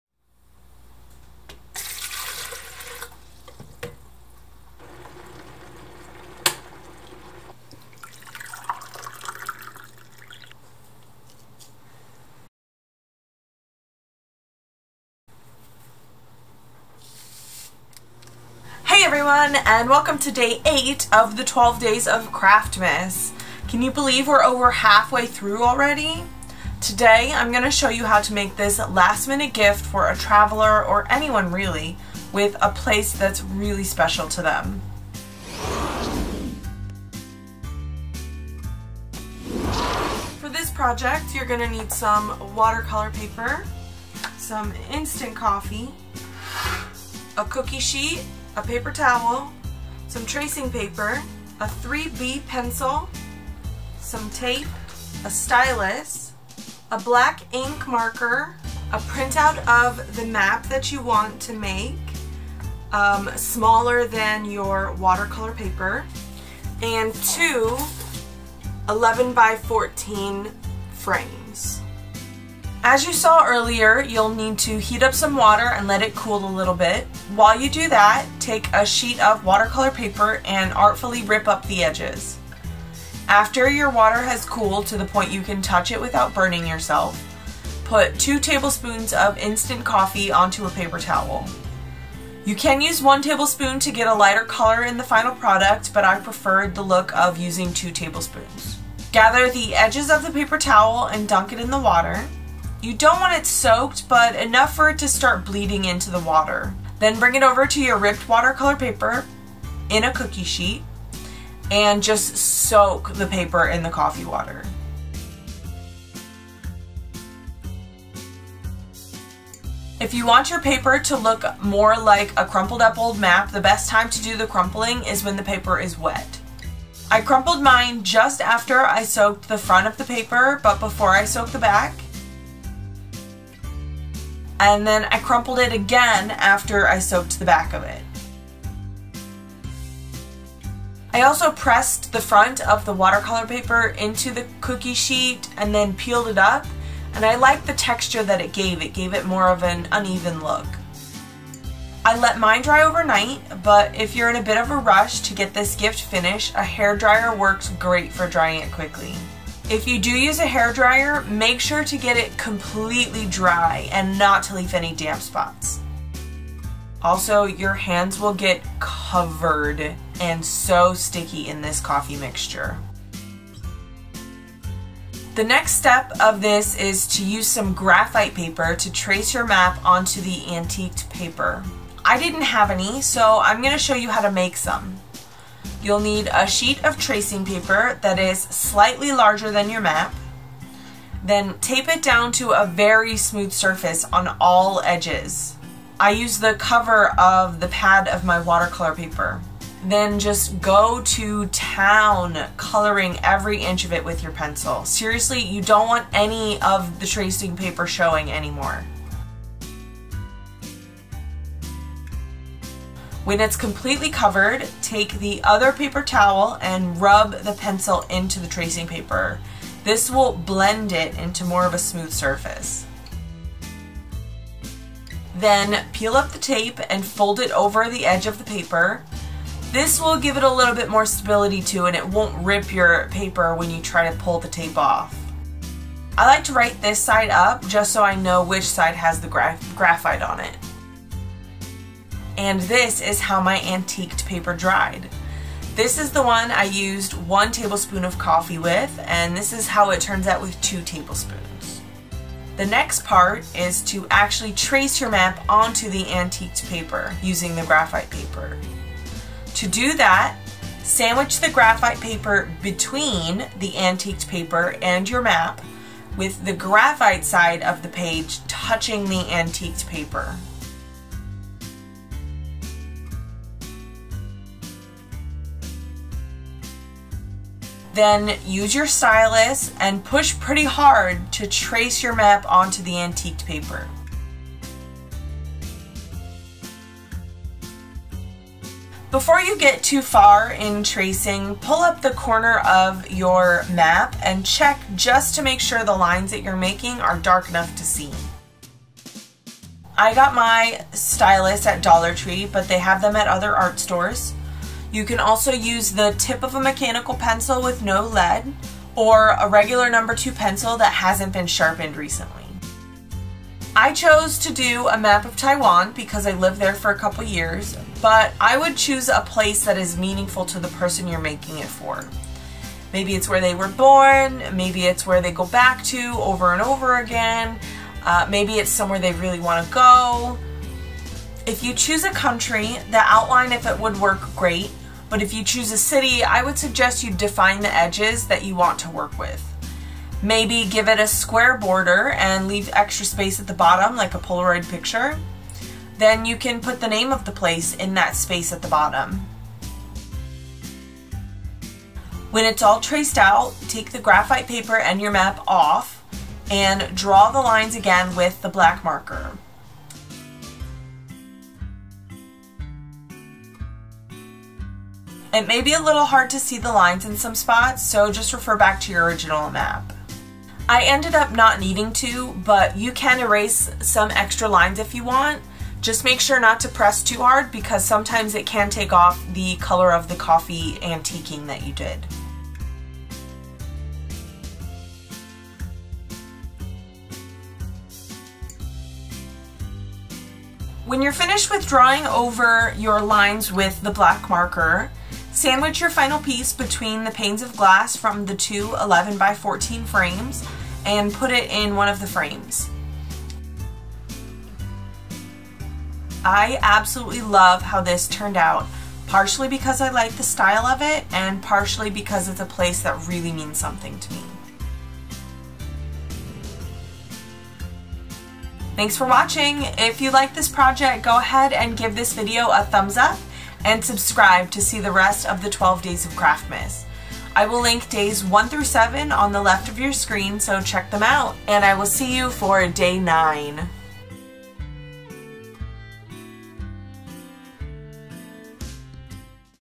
How to make an ANTIQUE MAP with instant coffee | Graphite Transfer Paper DIY | 12 Days of Craftmas
Today's craftmas project is how to make an antique map with instant coffee! I really love how it turned out. I'm using multiple techniques while making this including how to make antique paper with instant coffee and a graphite transfer paper diy.

Also, I'm so sorry about the focus being off while I was tracing the map. I forgot to turn auto focus back on after doing some other photography.

SUPPLIES
Watercolor paper
Instant coffee
Cookie sheet
2 paper towels
Tracing paper
3b pencil
Tape
Stylus
Black marker
Printout of map
2 11X14" frames

CHAPTERS
0:00 Intro
0:40 Supplies needed for how to make an antique map with instant coffee
1:13 How to make antique paper with instant coffee
2:54 Graphite transfer paper DIY
4:13 How to use graphite transfer paper
6:30 how to display an antique map on a wall
6:43 DIY antique map